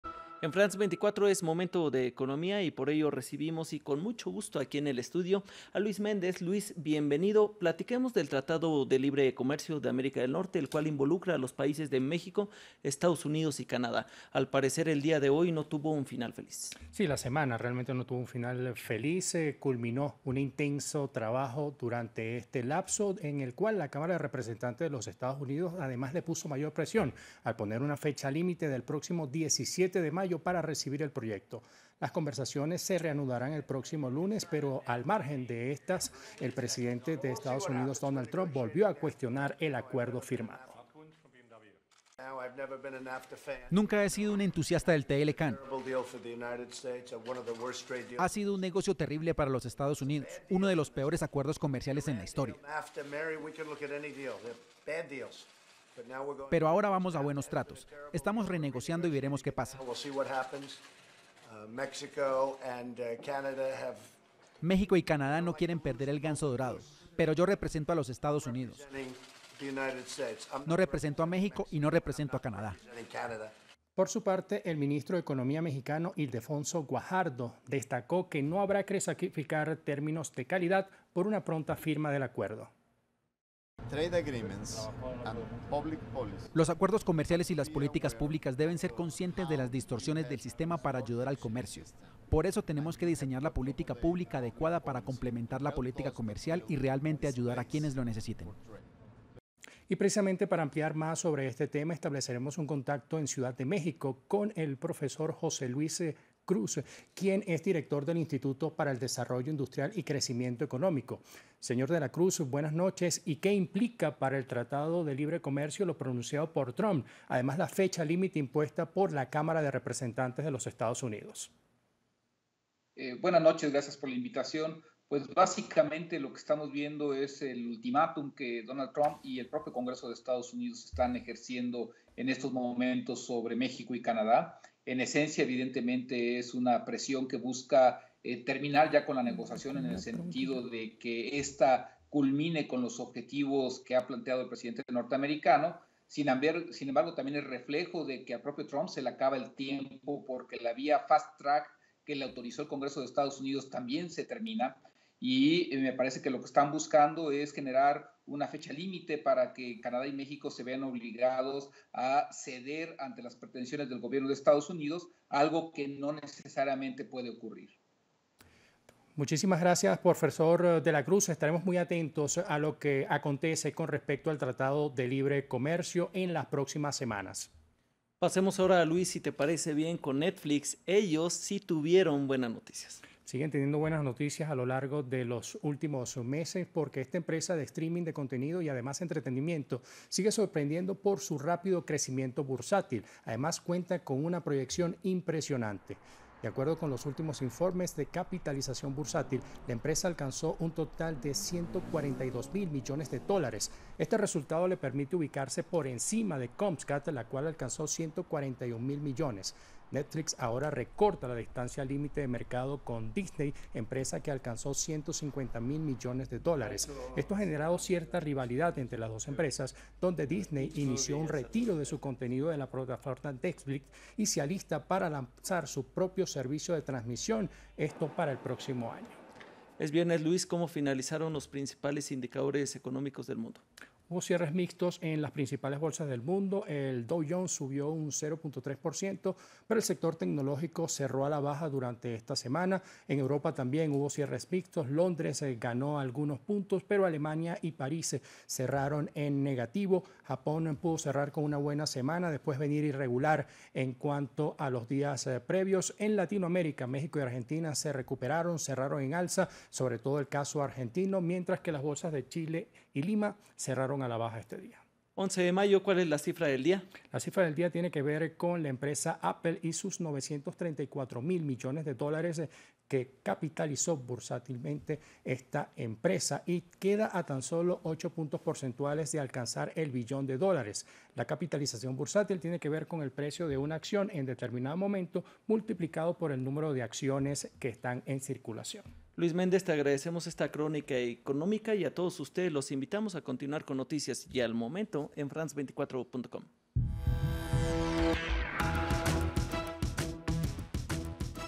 Las conversaciones por el TLCAN siguen sin llegar a acuerdos concretos
La semana culminó sin que los representantes de los países involucrados en el TLCAN llegaran a acuerdos concretos. Aunado a ello, el Congreso de Estados Unidos anunció que la fecha límite para recibir el proyecto será el 17 de mayo.